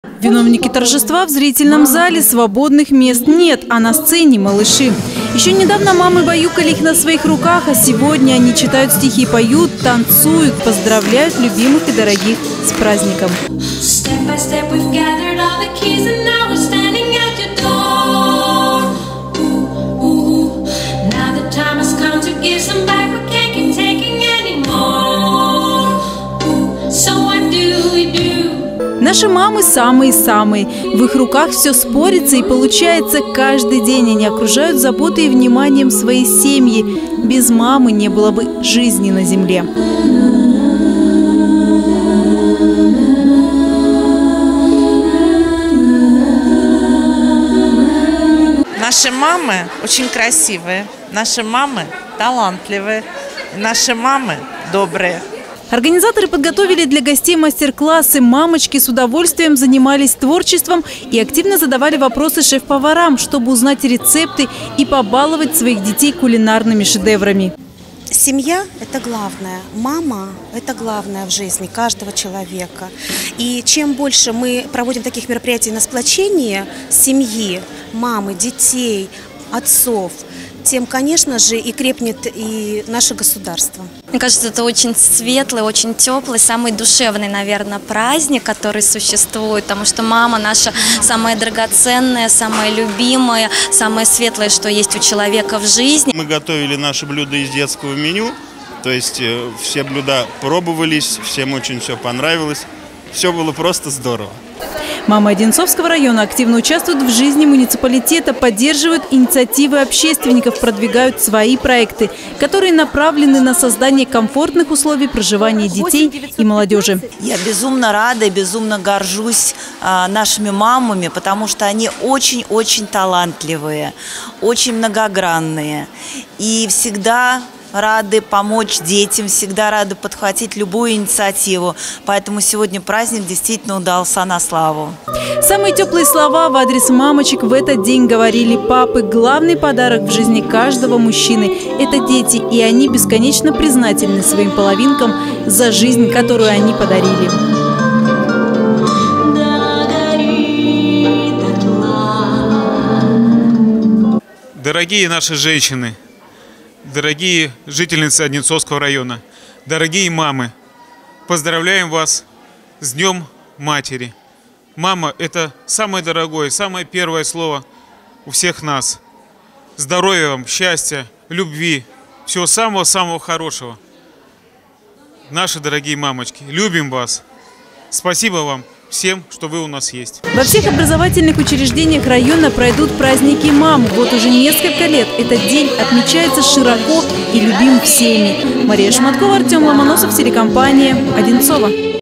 День матери в Одинцово
В культурно-спортивном центре «Мечта» прошло торжество по случаю Дня матери.  Организаторы подготовили концерт, разнообразные мастер-классы и шоу-программу. Прекрасных дам окружили вниманием и заботой.